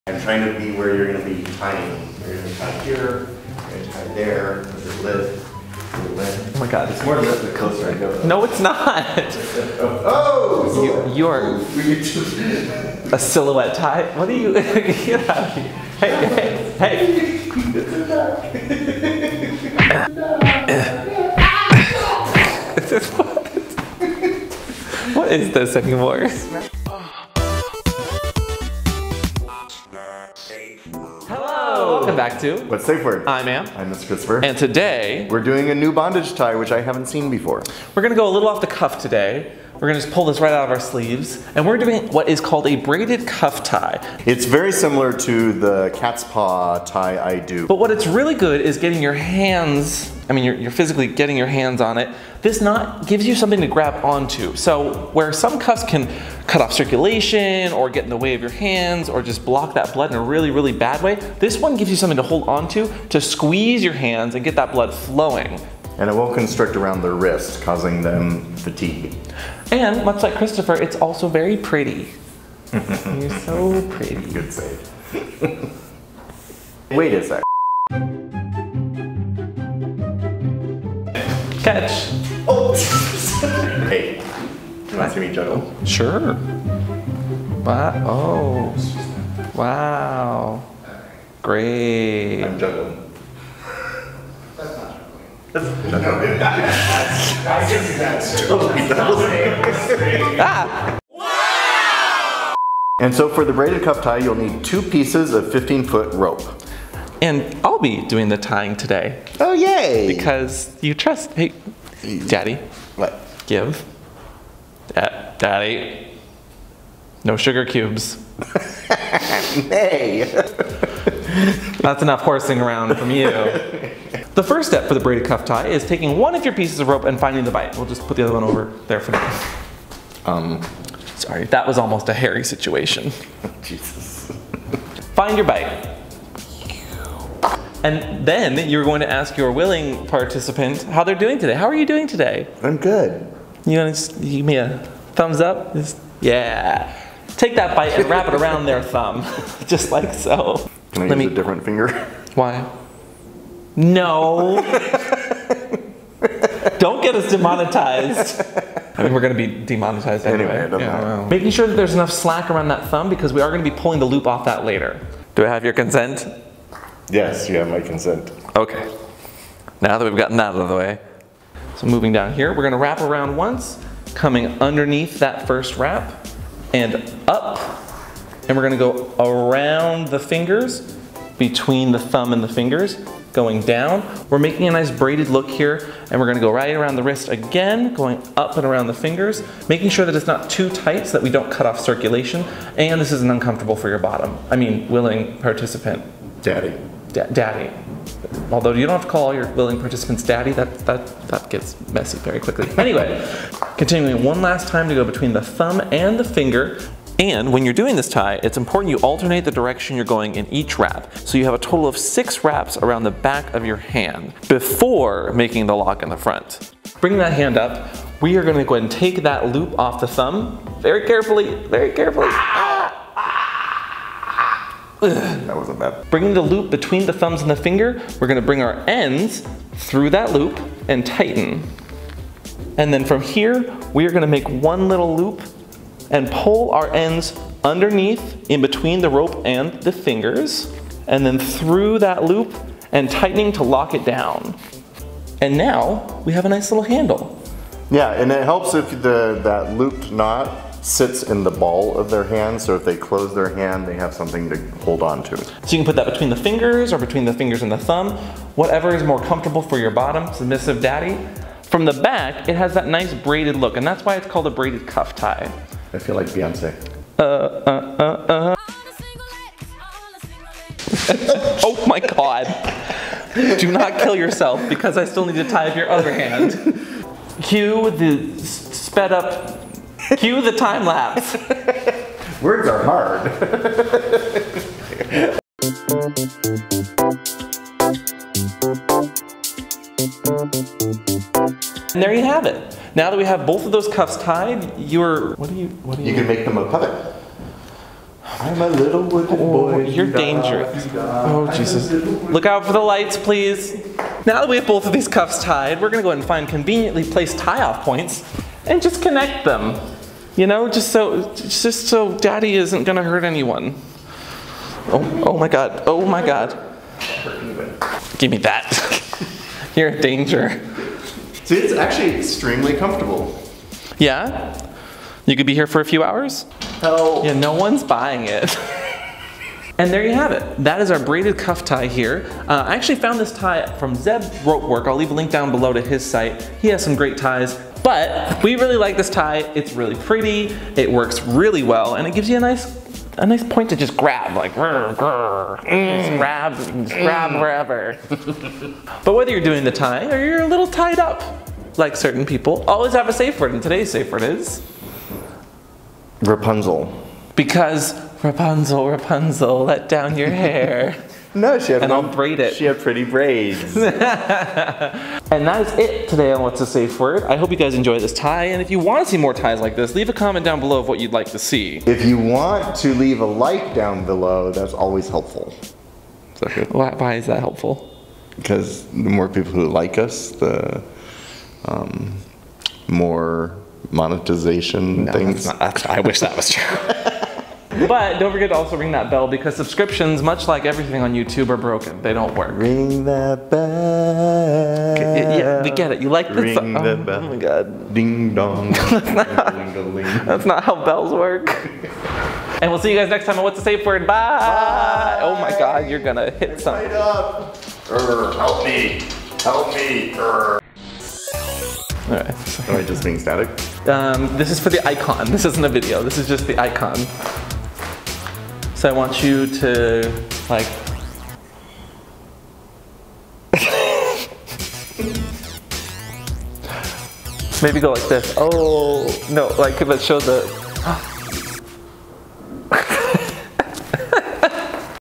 BRAIDED CUFF TIE - Bondage Tutorial
This one was something we did off the cuff, pulled out of our sleeve, and just generally enjoyed~ Let us know if you enjoy these bondage ties and if you have any ideas for what we should tie next time!